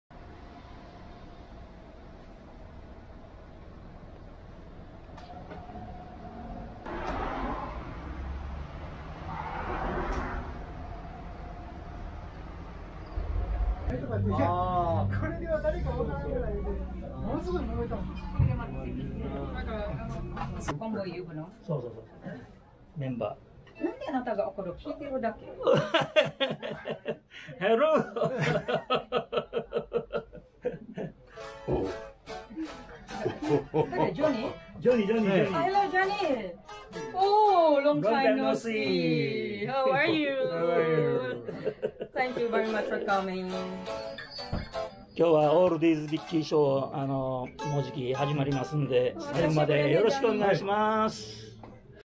ザ・オールディーズビッキー & ザ・アンティクス オールディーズライブ（2022.7.10 森のくまさん)楽屋風景
ザ・オールディーズビッキー
ザ・アンティクス
森のくまさん